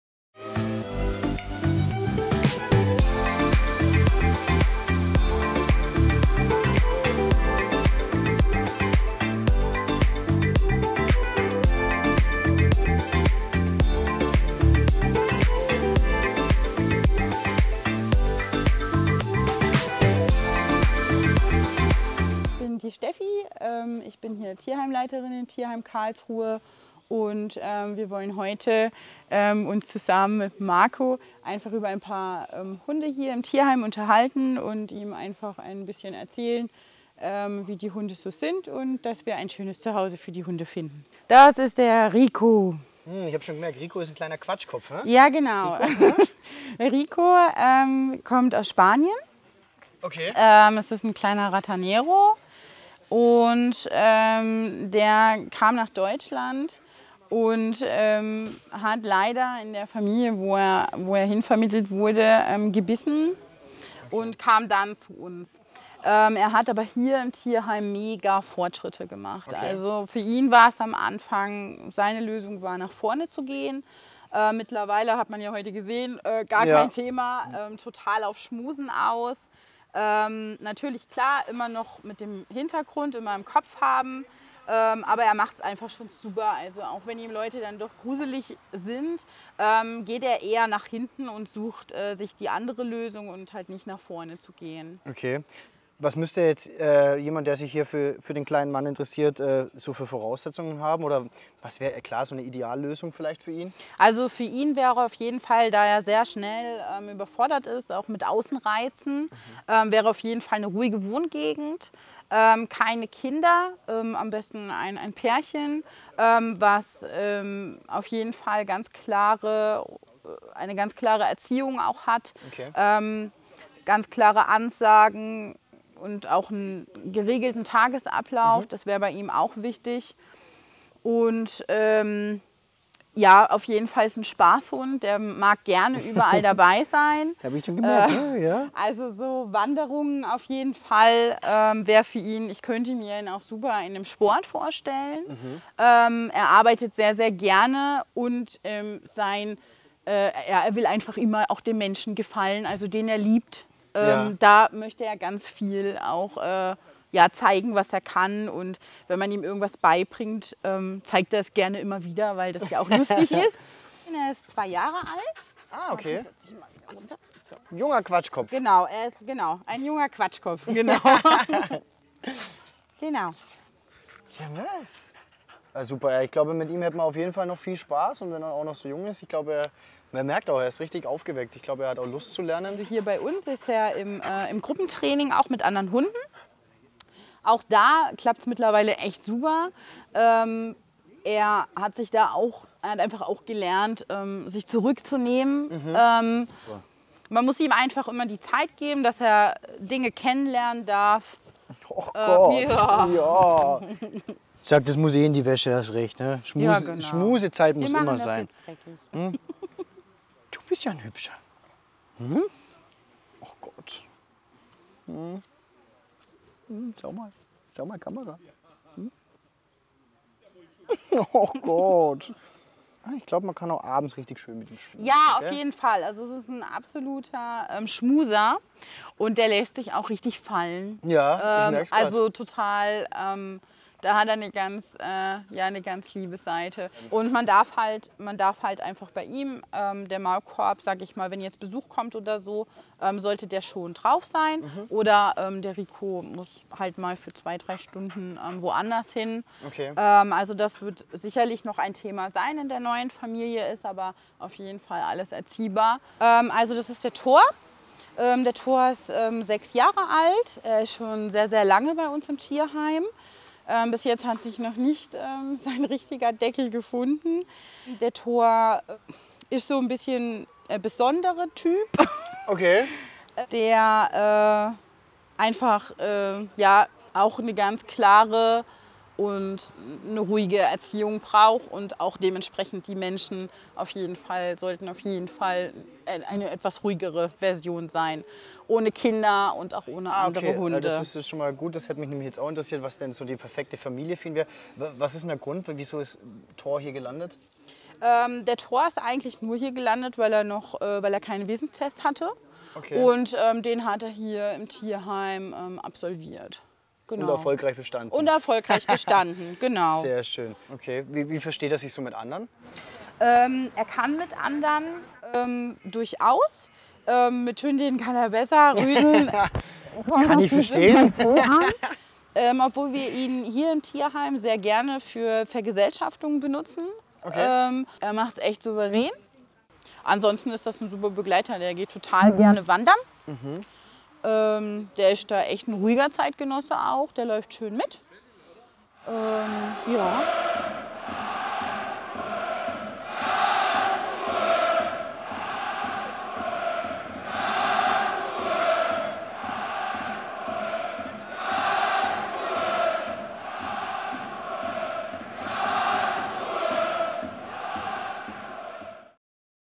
KSC TUT GUT | Hundevermittlung
Gemeinsam mit dem Tierschutzverein Karlsruhe u. Umgebung e.V. stellen wir im Rahmen von KSC TUT GUT. zusammen mit Marco Thiede ein paar Hunde vor, die ein neues Zuhause oder Unterstützung suchen.

Bei Interesse an einem Hund wendet euch direkt an:
oder unter folgender Telefonnummer: 0721/950780